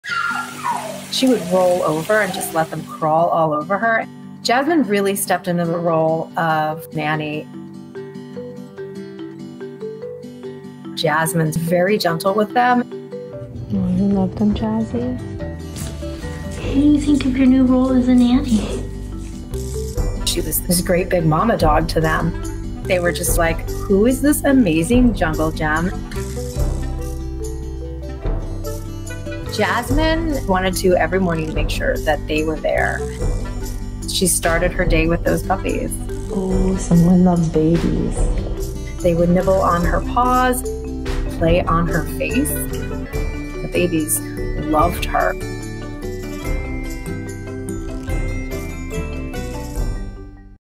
100-Pound Dog Is The Perfect Nanny To Tiny Beagle Puppies | The Dodo
This pittie is the best second mom!

Special thanks to Butter, Jasmyne, Elli & Mr. Bones & Co! Follow Elli on Instagram and Mr. Bones & Co. on Instagram